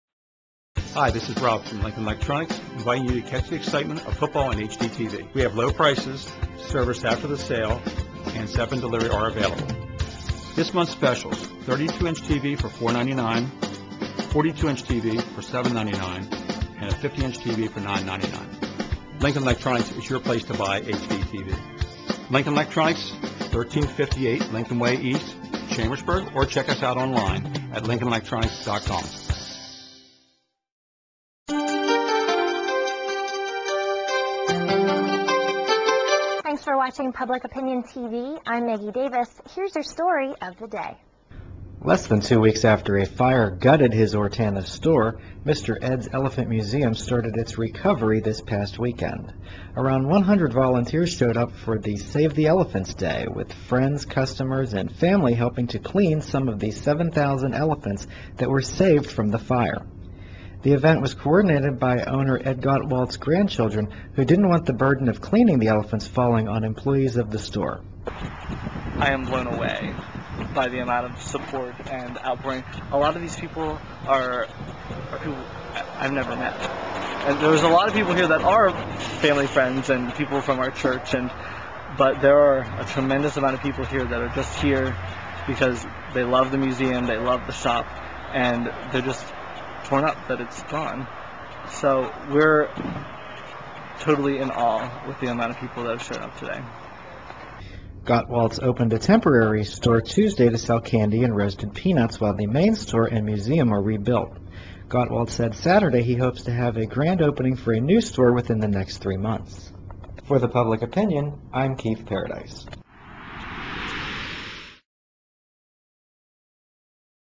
They turned out in droves to wash elephants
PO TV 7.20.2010